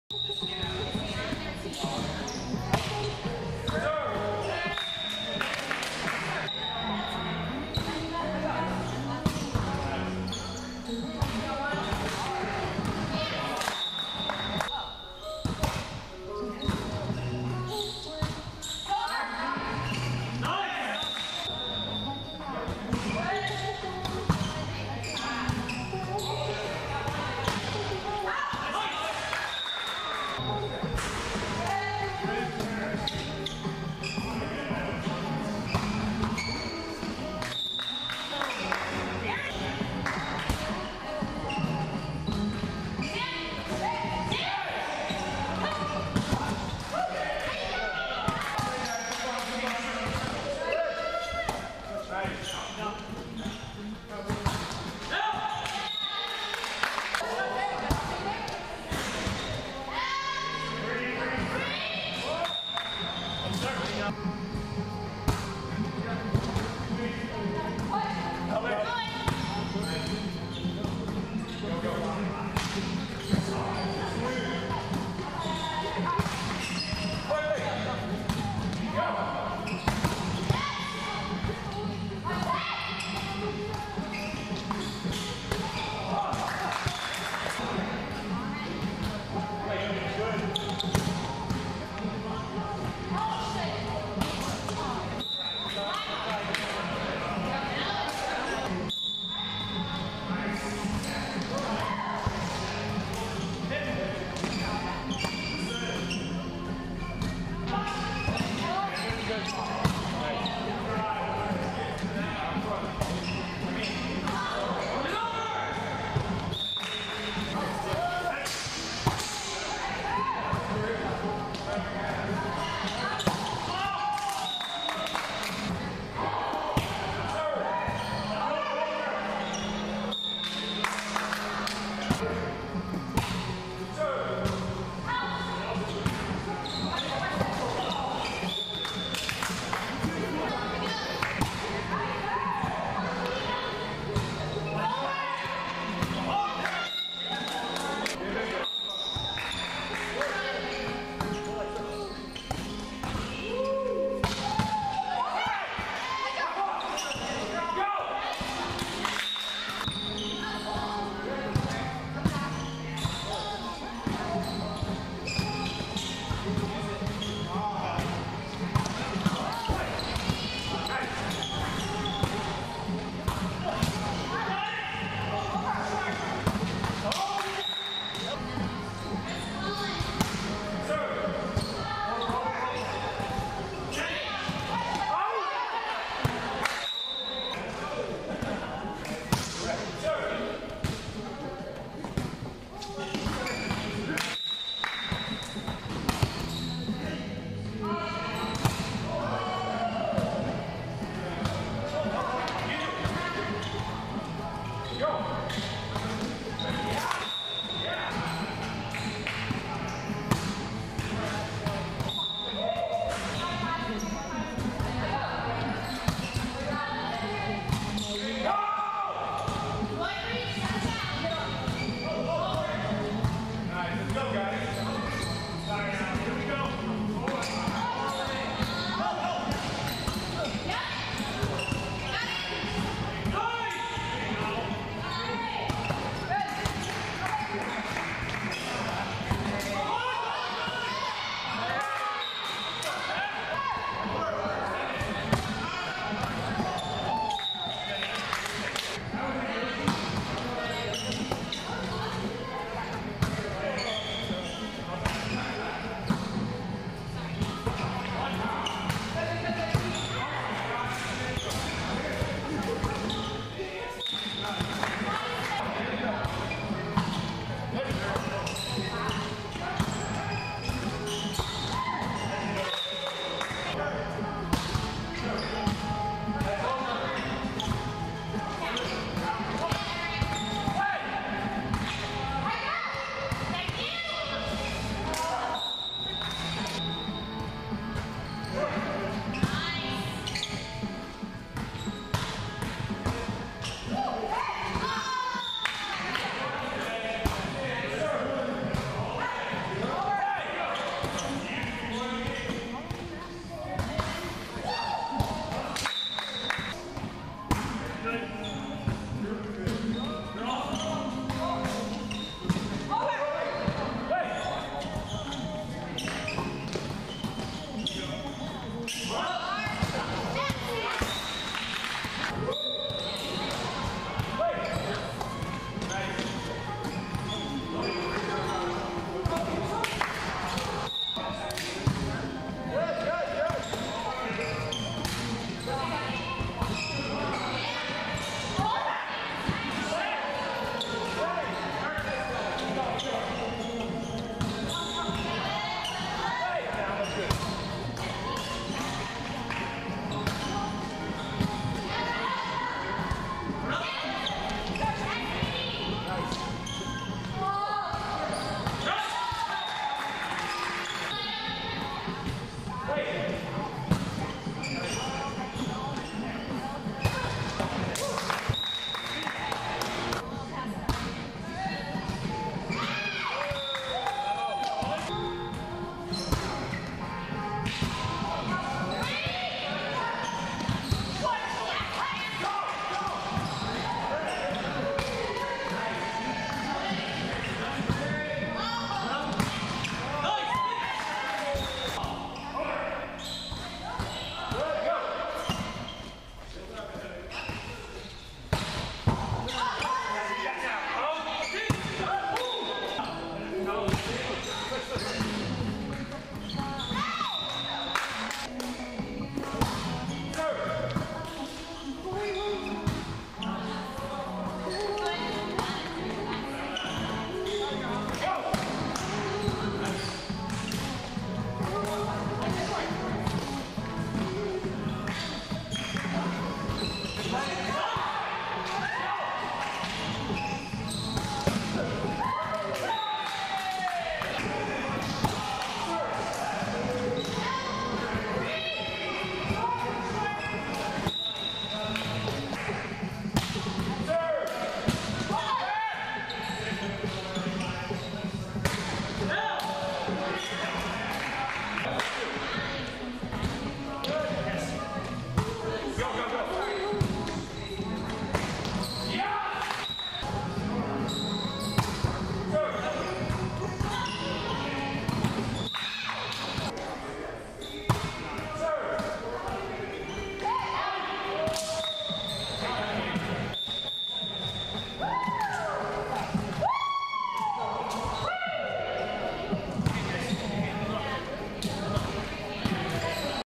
No Forfeit Tonight vs Shenanigans Game 2, Aug 31, 2022 NoIdeaSports
No Idea Sports indoor volleyball league at CJVA East Hanover facility